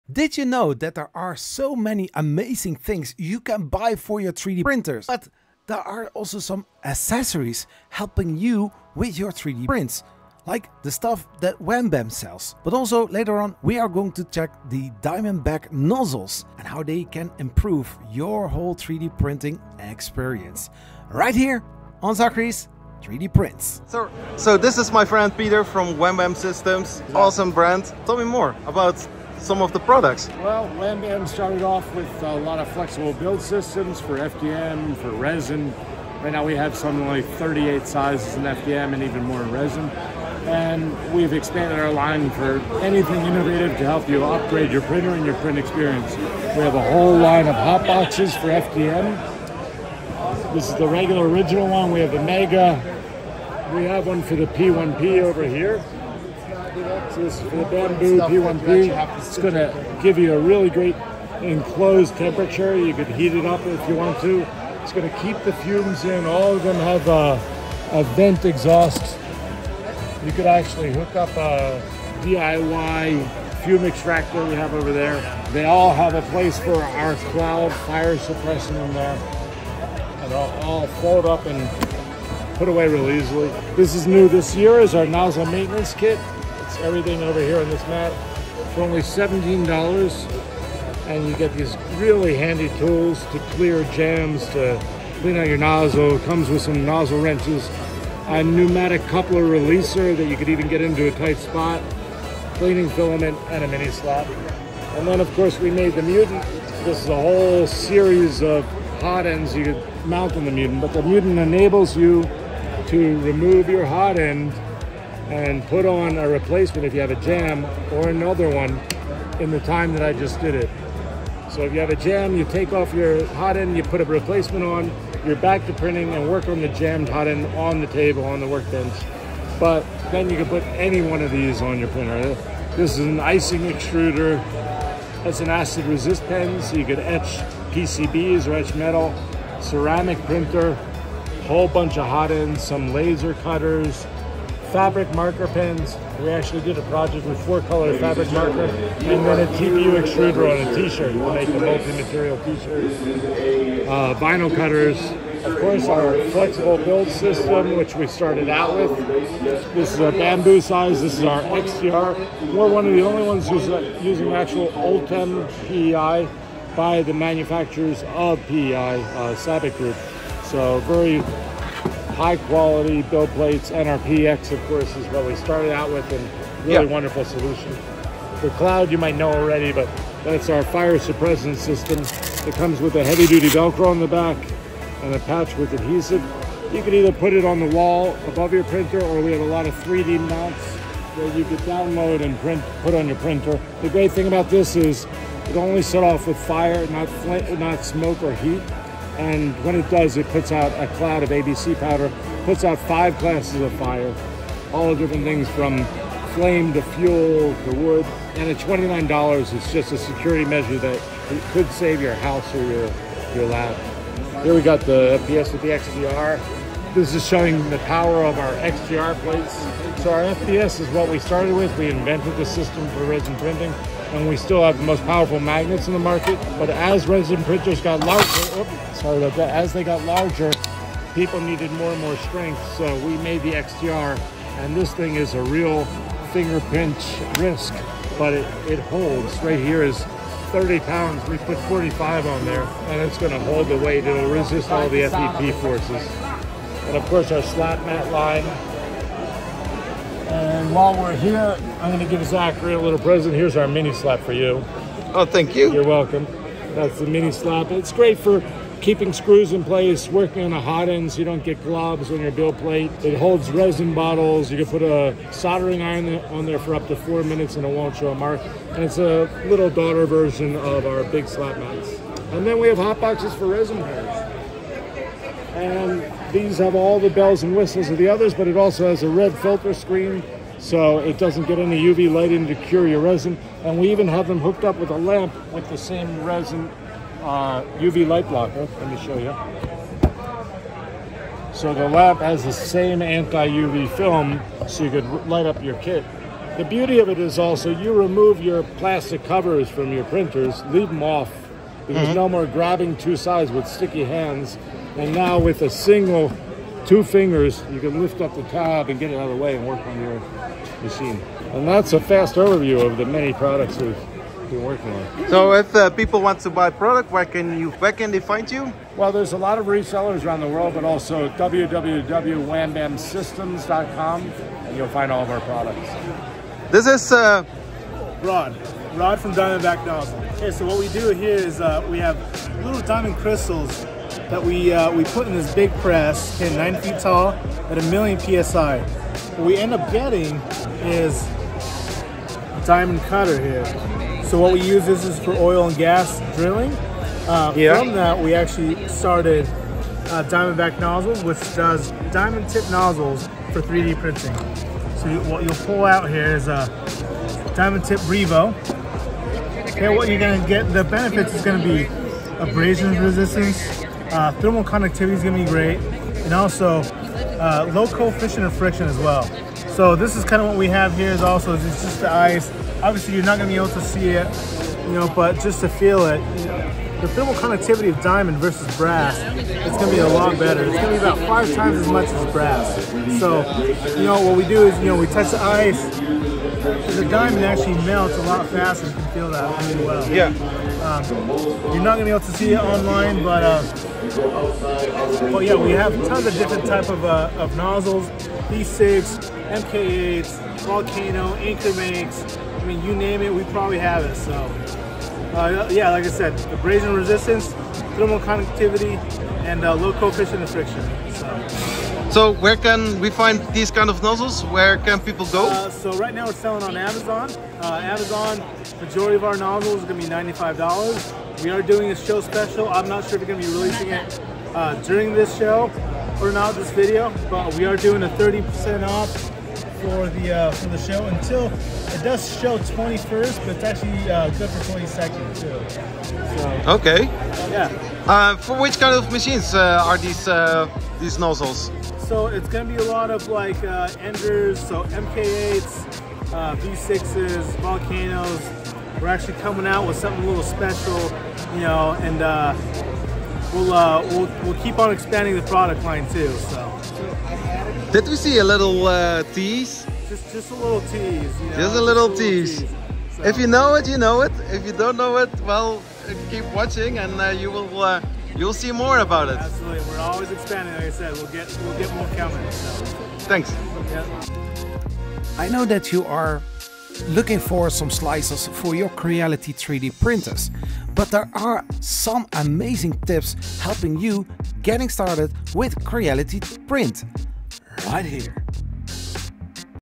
Improving your 3D Print Experience - WhamBam and Diamondback Nozzles - RMRRF2024
Learn how WhamBam Systems and Diamondback Nozzles can enhance your 3D printing experience in this RMRRF2024 video. Discover the benefits of the WhamBam HotBox and how Diamondback Nozzles can improve your prints.
Looking for some amazing upgrades for your 3D Printer or 3D printing experience? Check this video out, in this video I'll do some small interviews with Whambam systems and with Diamondback Nozzles and they show some cool additions for your 3D printers.

Diamondback Nozzles: